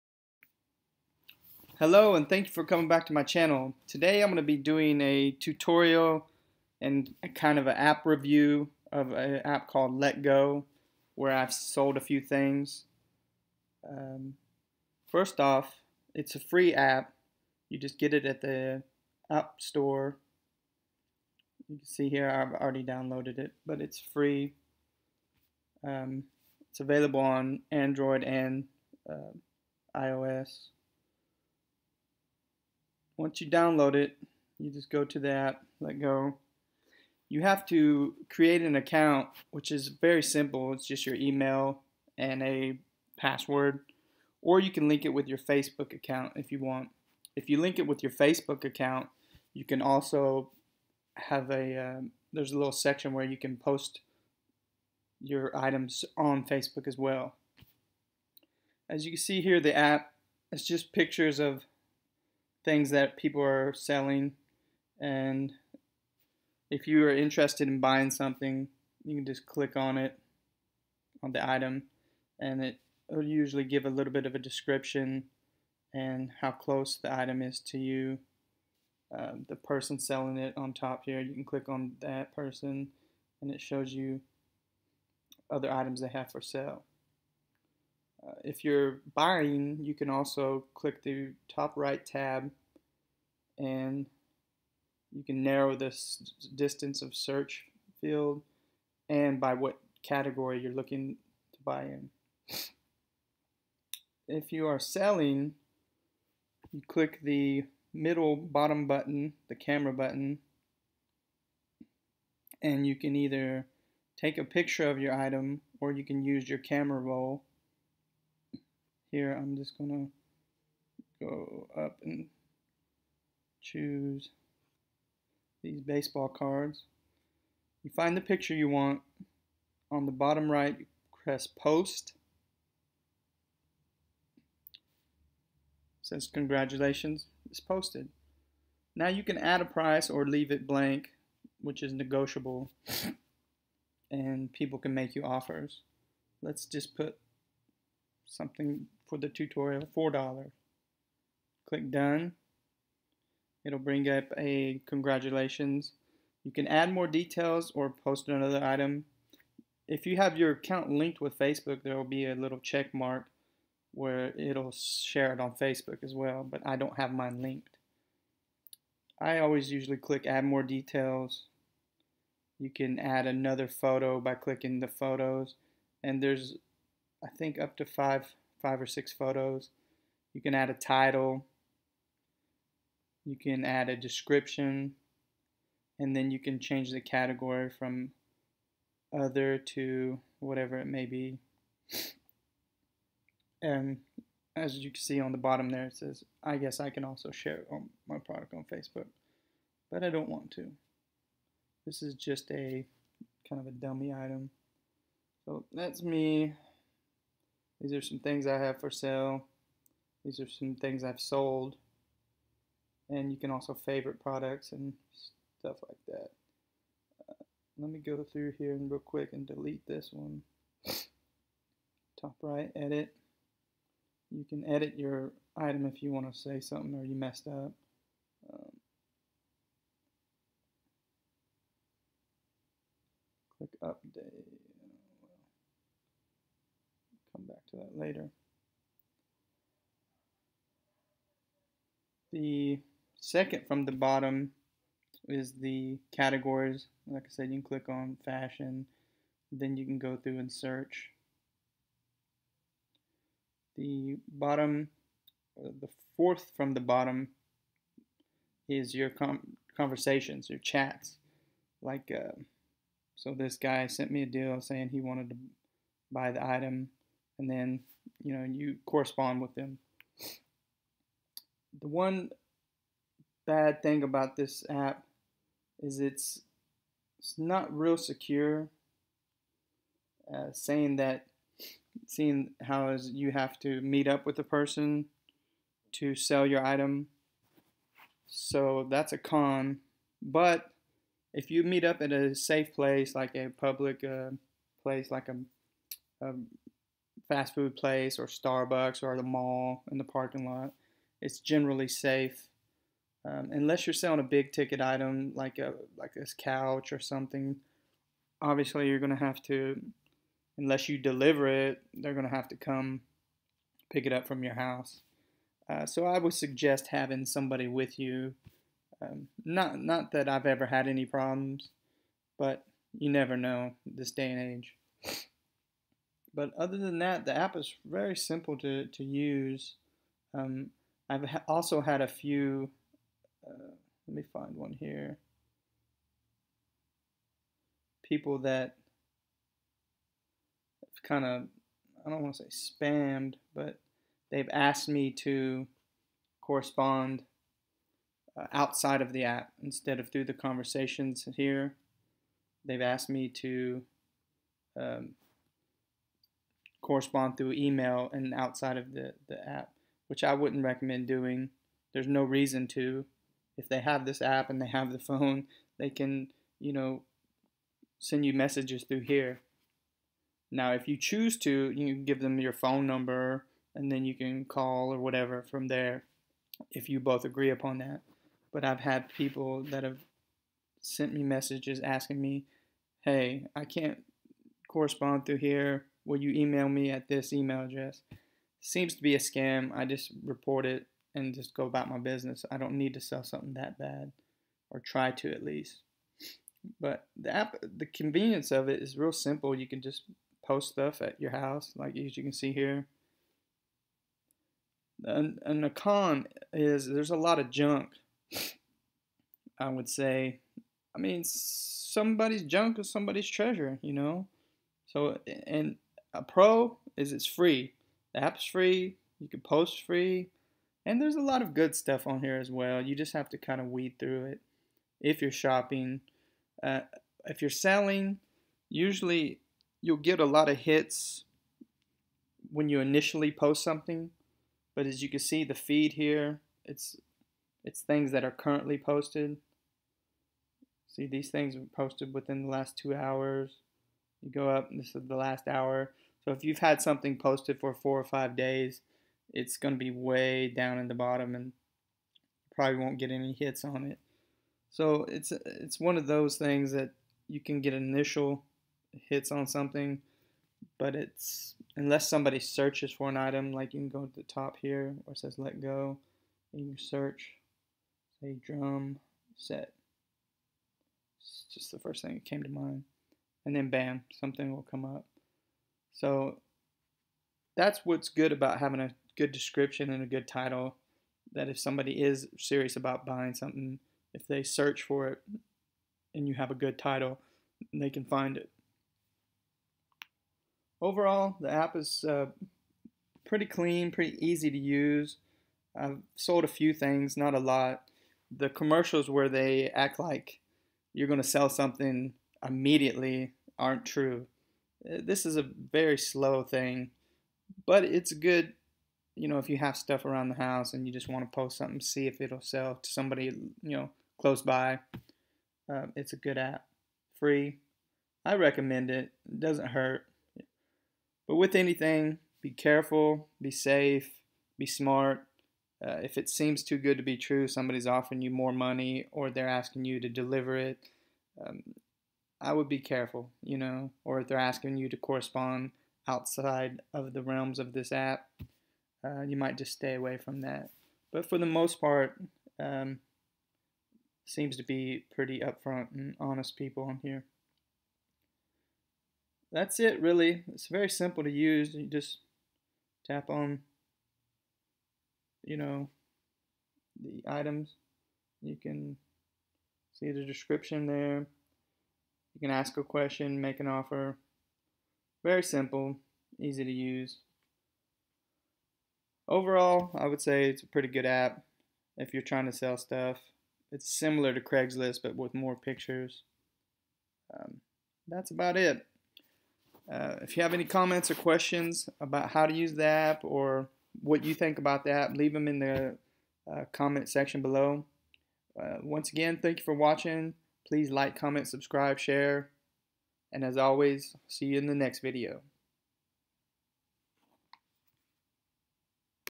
Letgo App Review & Tutorial
I show how to buy and sell on the app Letgo, available on IOS and Android. If you've used the app tell me what you think of the app in comment section or if you have questions please comment them.